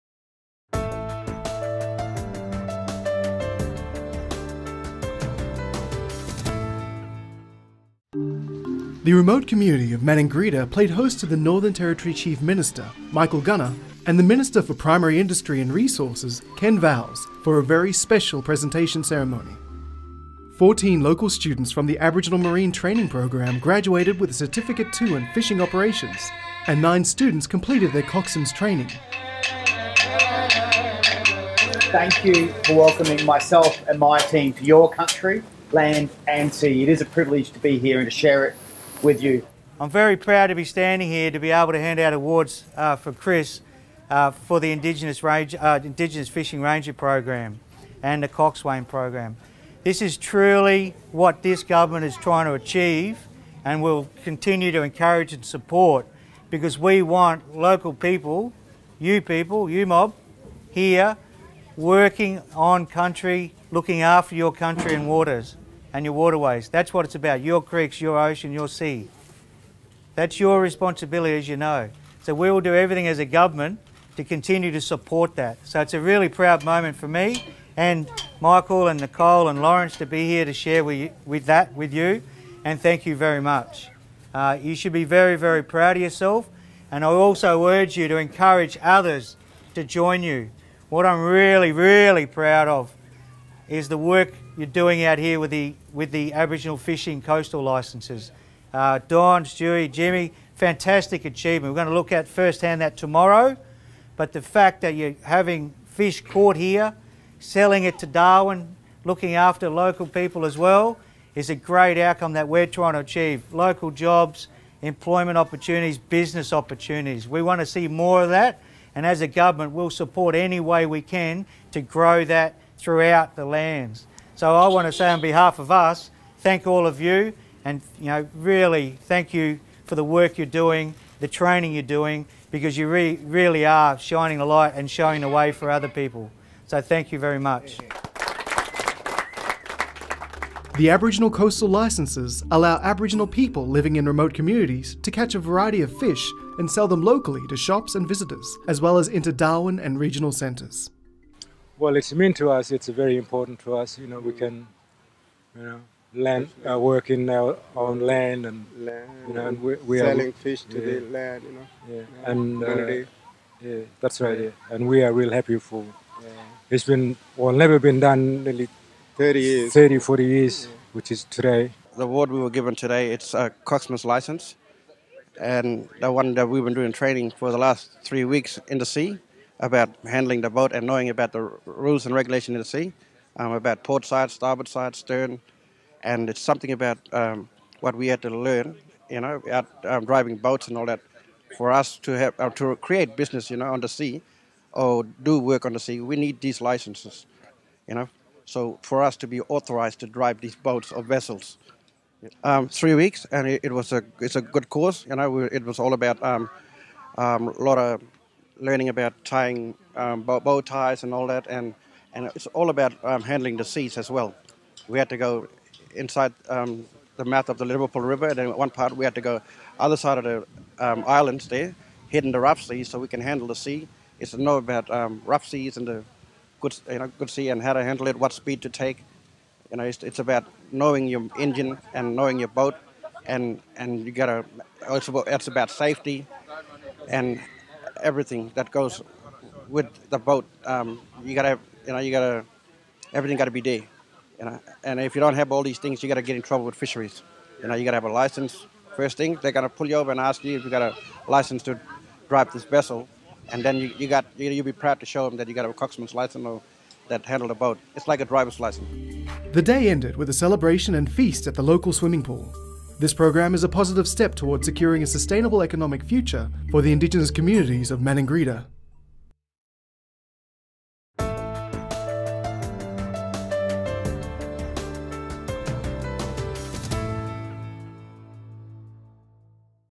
Maningrida Aboriginal Marine Training Program Graduation
The remote community of Maningrida played host to the Northern Territory Minister for Primary Industry and
Resources, Ken Vowles and the Chief Minister Michael Gunner for a very special presentation ceremony.

14 local students from the Aboriginal Marine Training Program graduated with a certificate 2 in Fishing Operations and 9 students completed their Coxwains training.